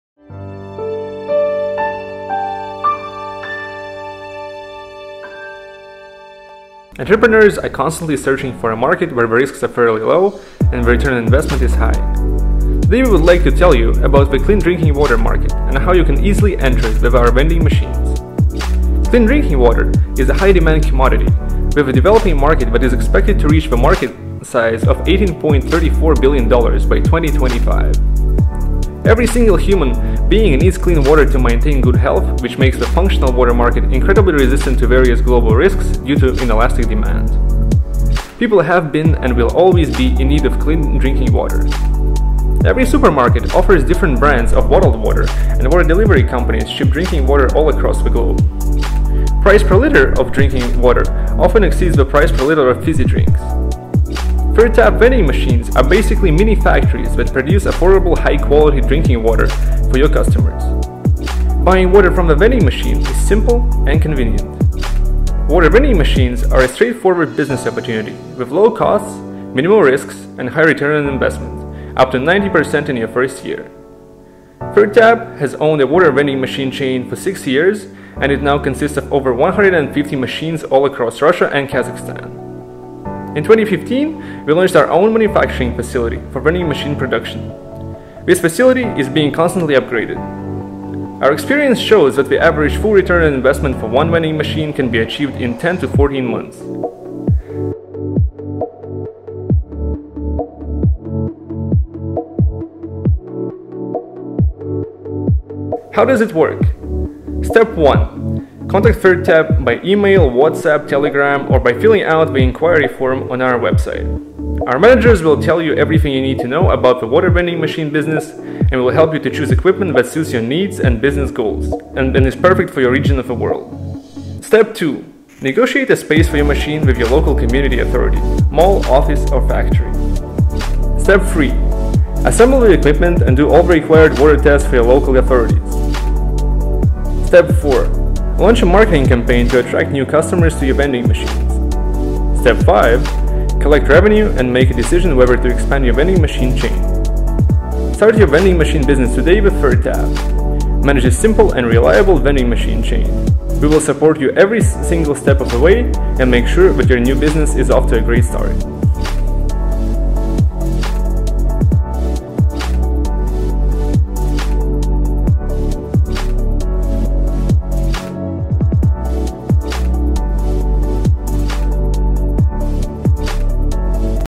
How to start your water vending machine business with the equipment made by Third Tap.
In this video we are going to tell you why water vending machine business is an extremely viable option for an entrepreneur right now and how you can start it using the equipment that we produce at Third Tap LLC. You can find out more about our products on our